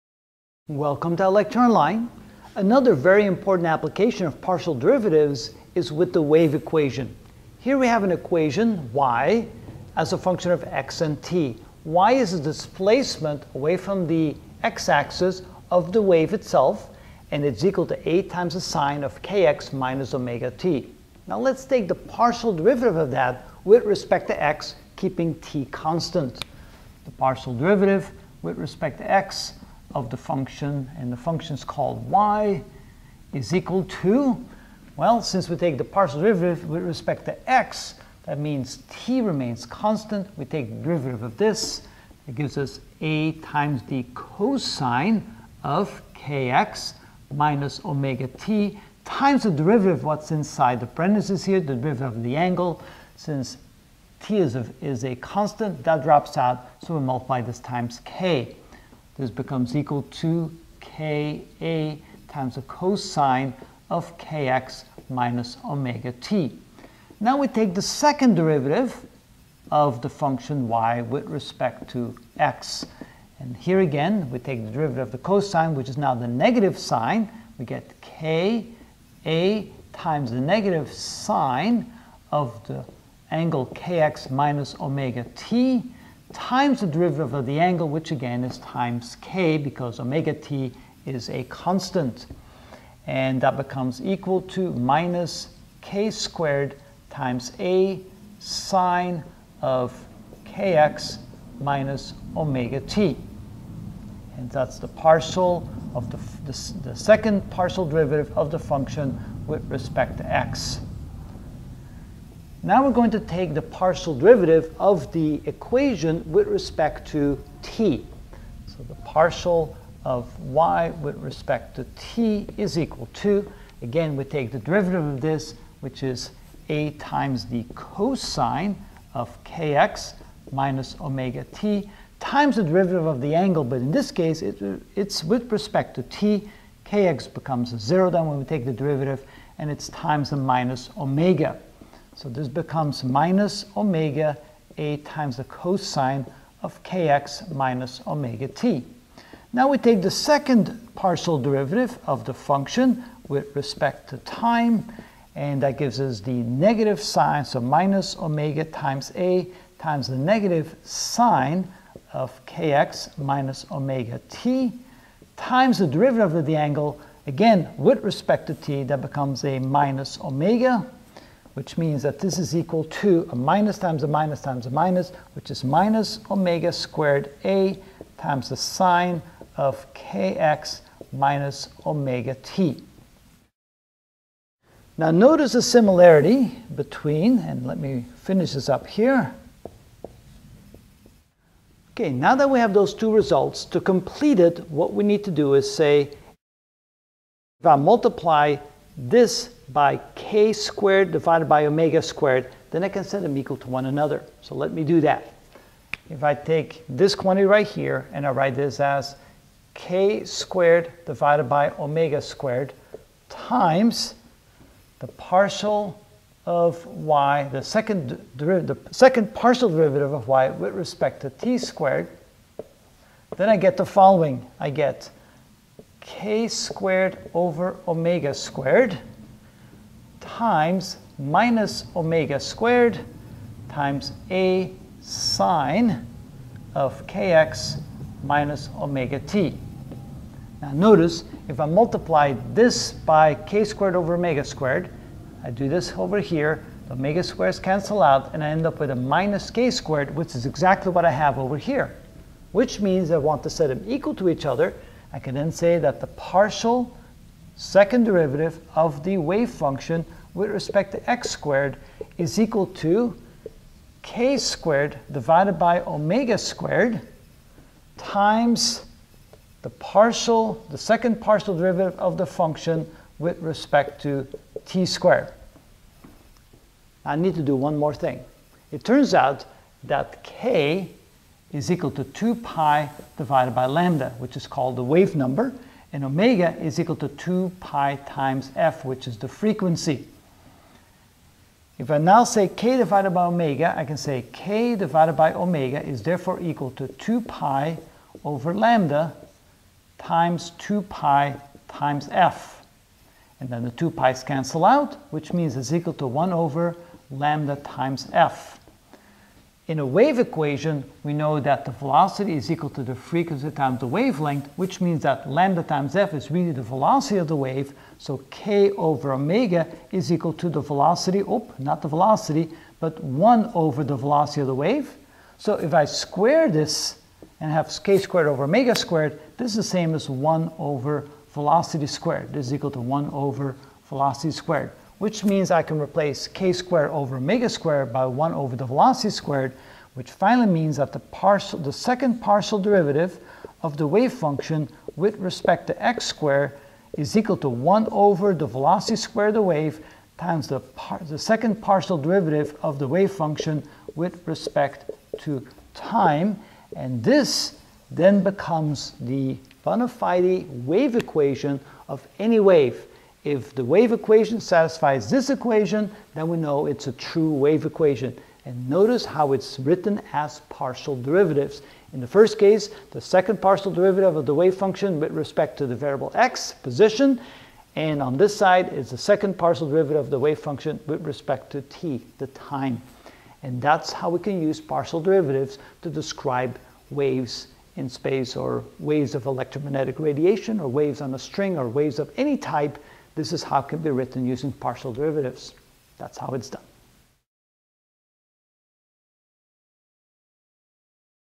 Calculus 3: Partial Derivative (16 of 30) Application of Partial Derivatives: The Wave Equation
In this video I will use the 1st and 2nd partial derivative with-respect-to x and t to find the wave equations.